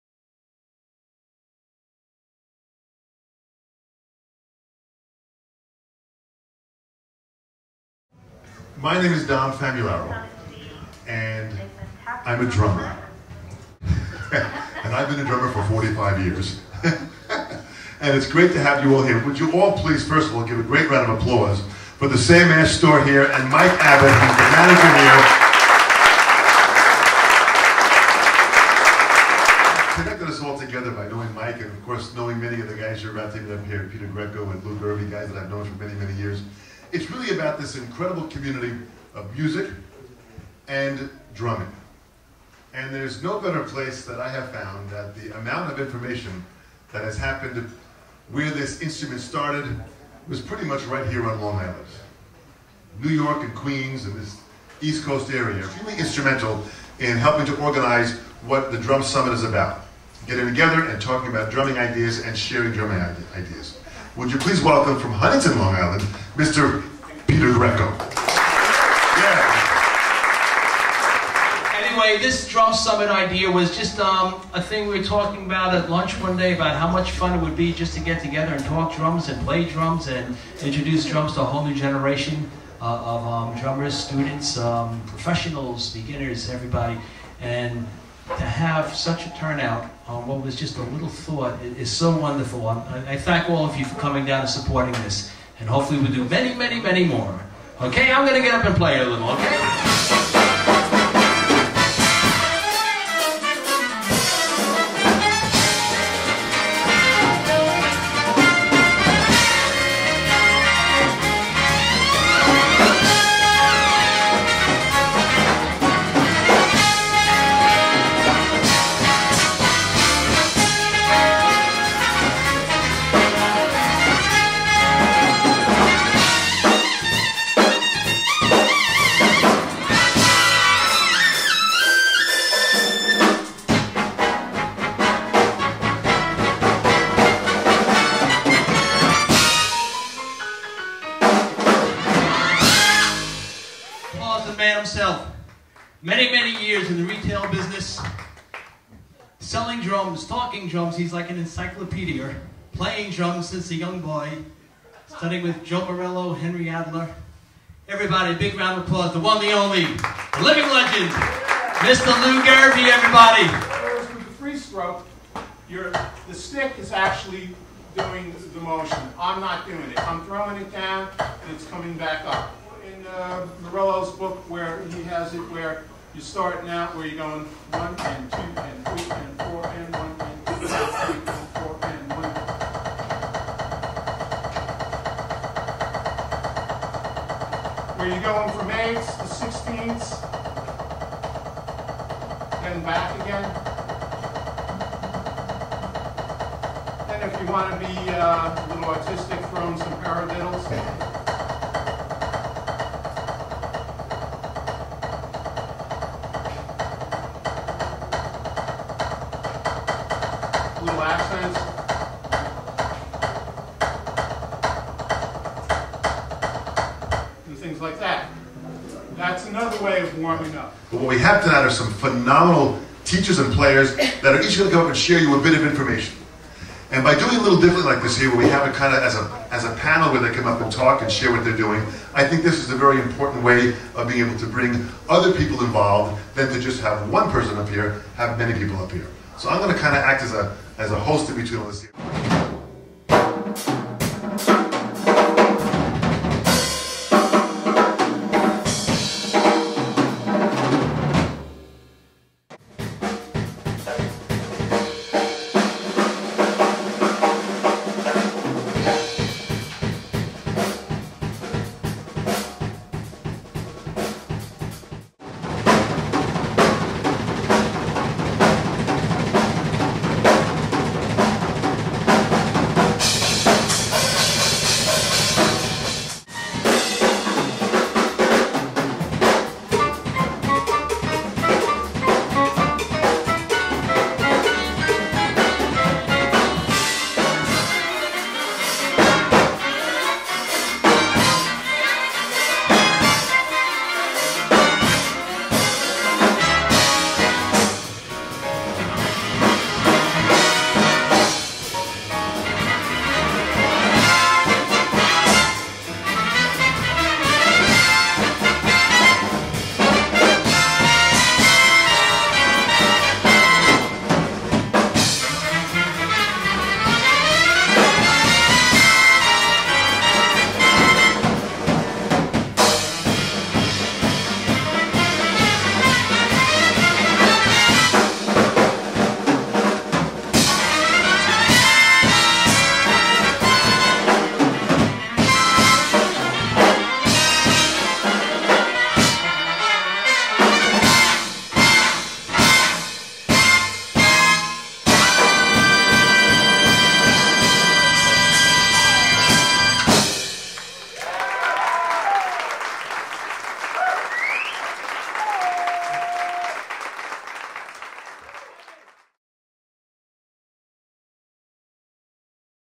The Drum Summit - Dom Famularo, Peter Greco & Lou Gervey
Dom Famularo, Peter Greco & Lou Gervey present - The Drum Summit. The Ultimate Educational  Clinic